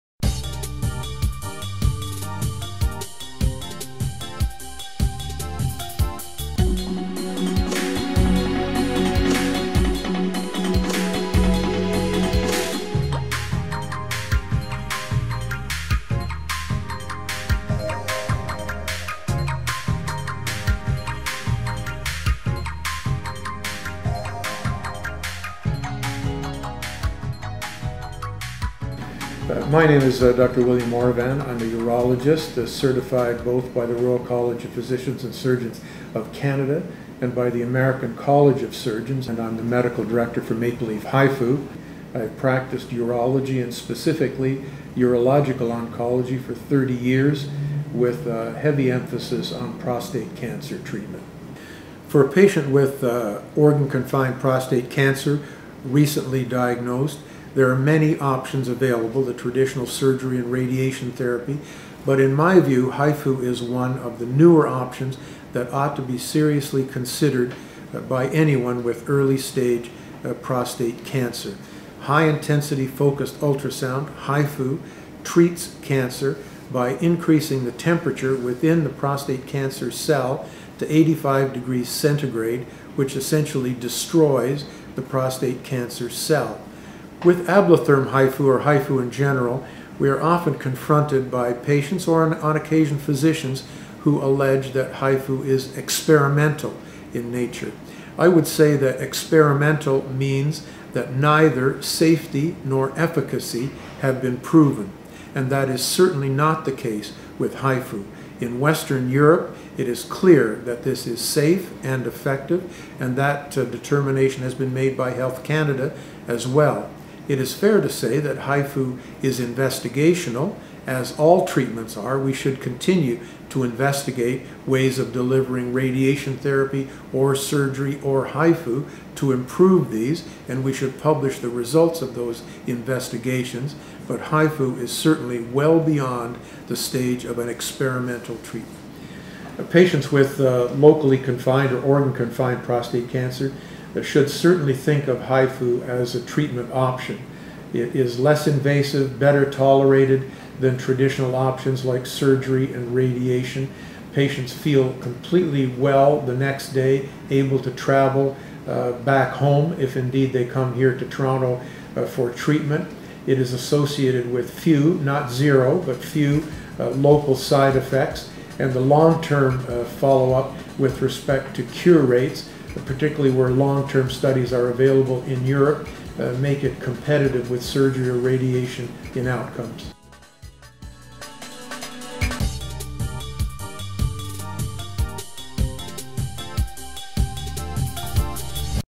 Prostate Cancer New Treatment HIFU Toronto
Prostate Cancer New Treatment HIFU Toronto.  HIFU - or High Intensity Focused Ultrasound - is one of the newer treatments for prostate cancer.  It is a non-surgical, non invasive prostate cancer treatment that is as effective as surgery or radiation.  The first North American clinic to offer HIFU was the Maple Leaf Clinic in dowtown Toronto, Canada.  They have treated more men with organ confined prostate cancer than any other HIFU clinic in North America.  They are located in the Cleveland Clinic Canada and have been providing the Ablatherm HIFU treatment since 2005.